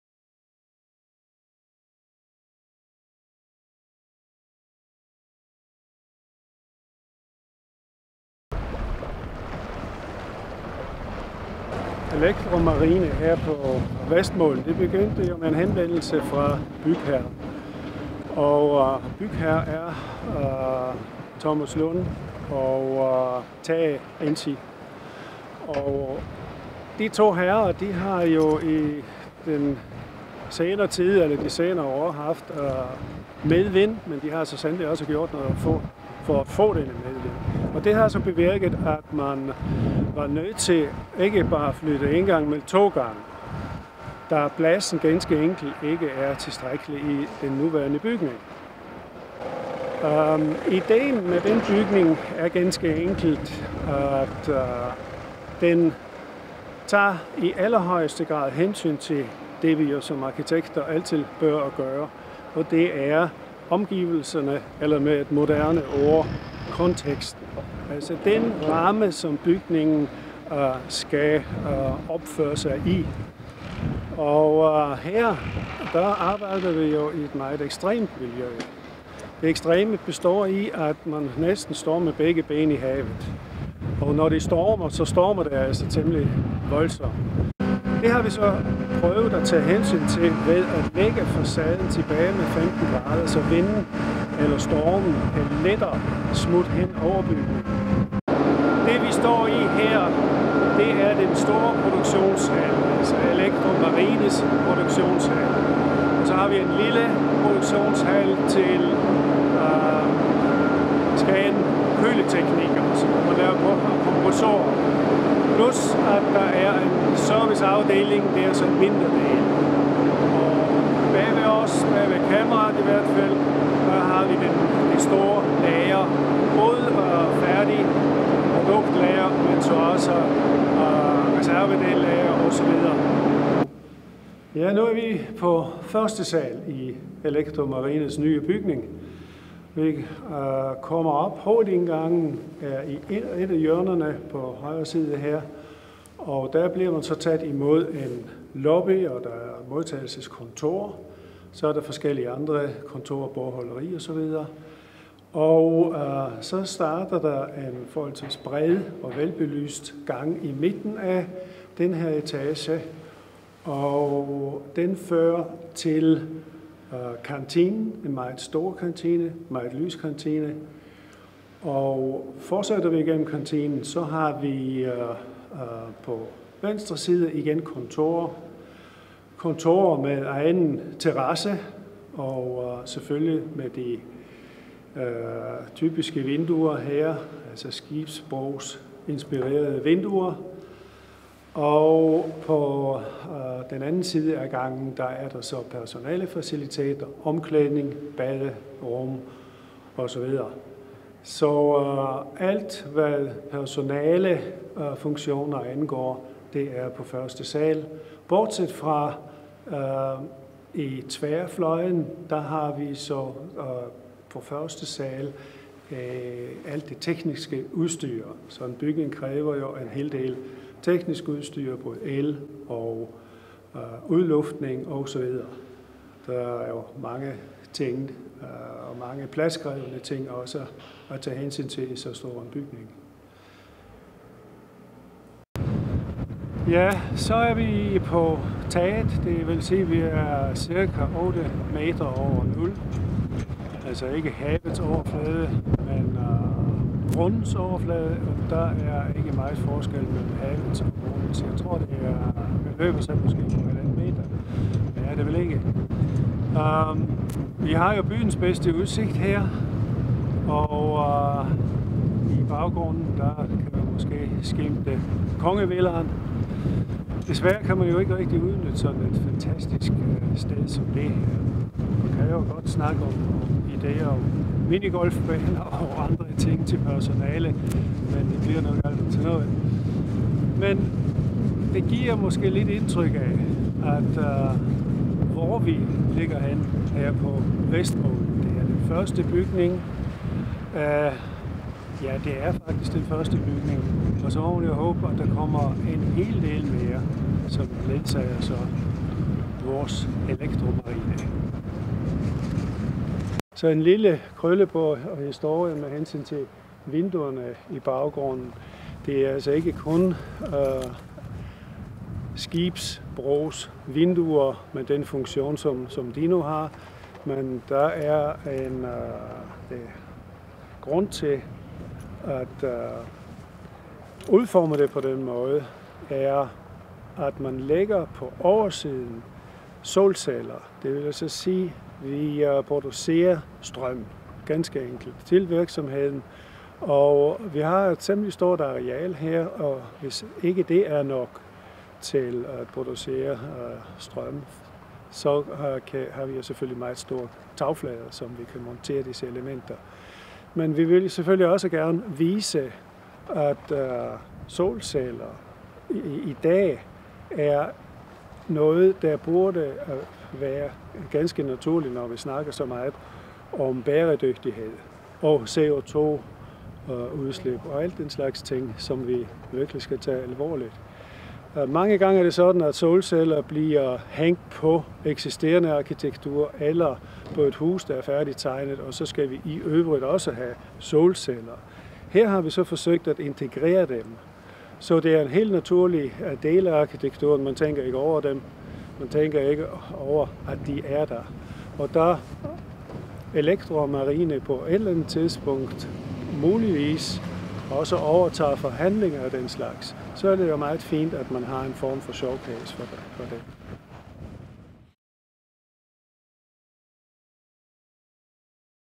elektromarine
Præsentation af Elektromarines nye fabrik som er under opførsel på Skagen havn. Præsenteres af arkitekten, Jörg Kerchlango